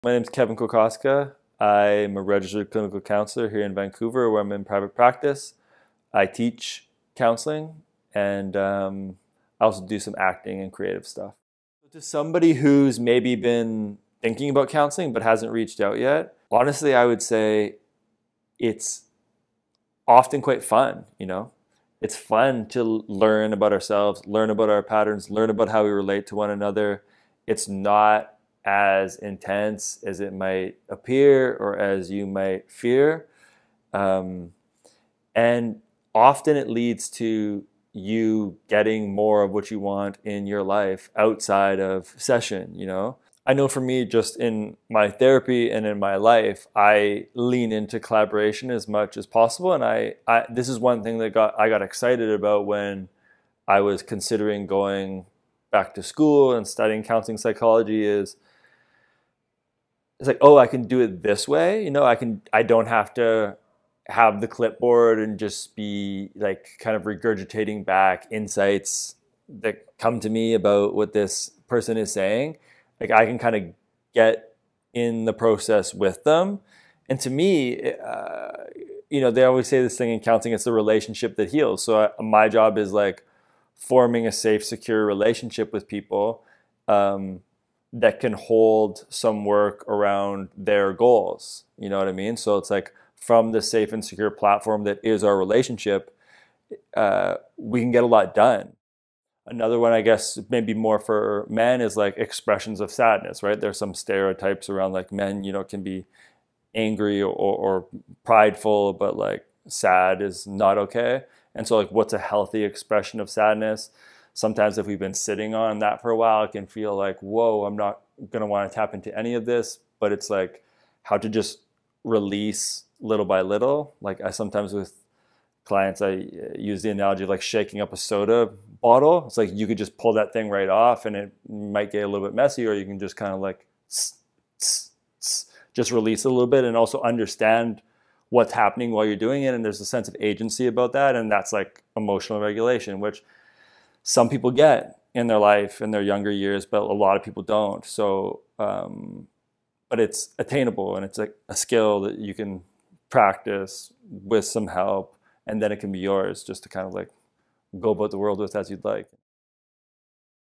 Kevin Kokoska - BC Clinical Counsellor
Kevin Kokoska is a Registered Clinical Counsellor, and psychotherapist in Vancouver.

Book a session with Kevin, or find another therapist that is right for you